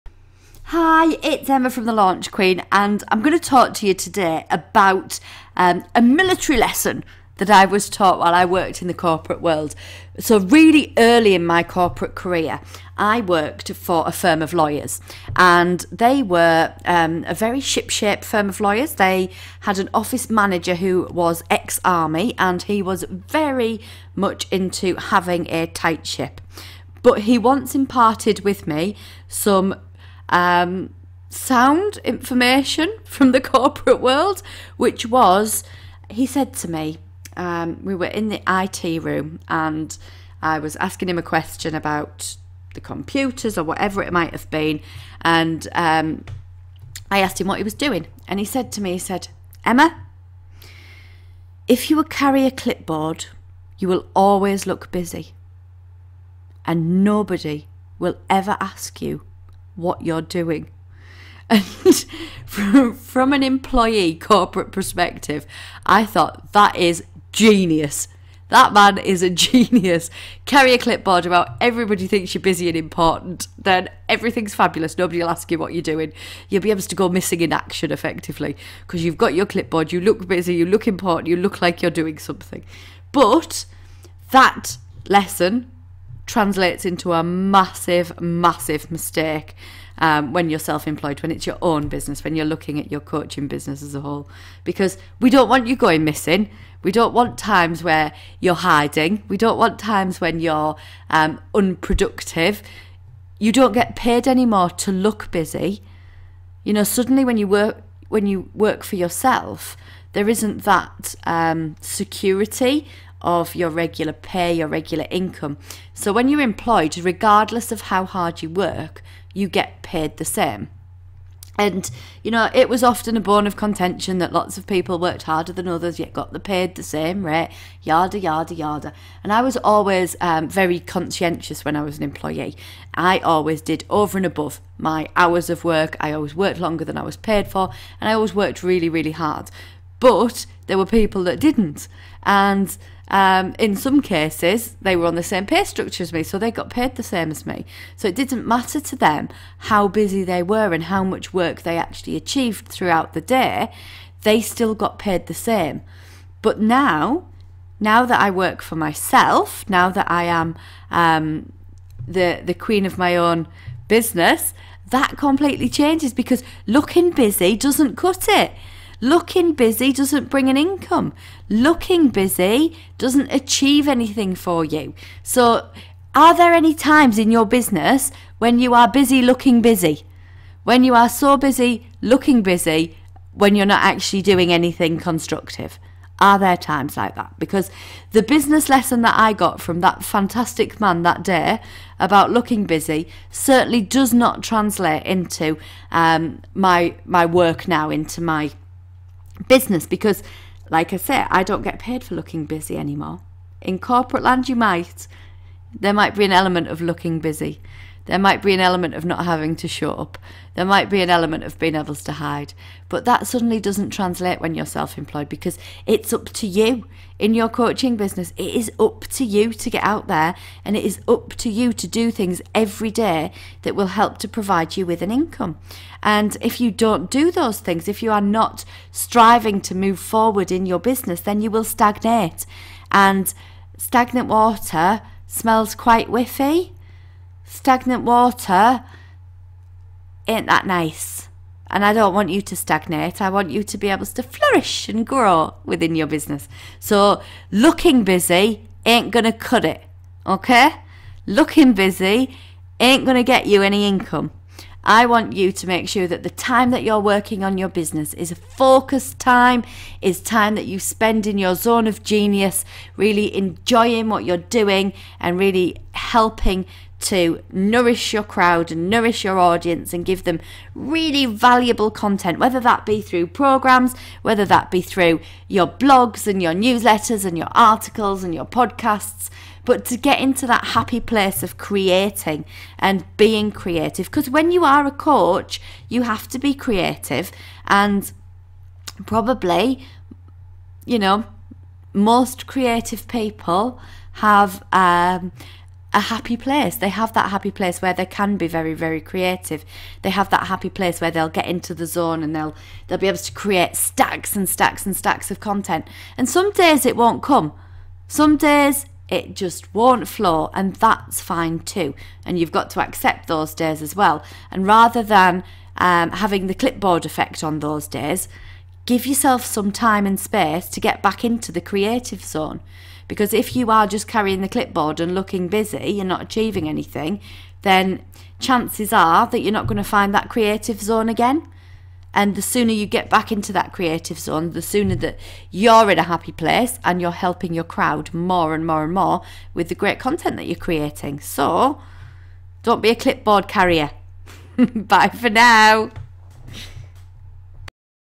This Big Gun Lesson Can Translate Into A Big Mistake
Find out how I was taught to look busy in the corporate world and how that doesn't translate into business!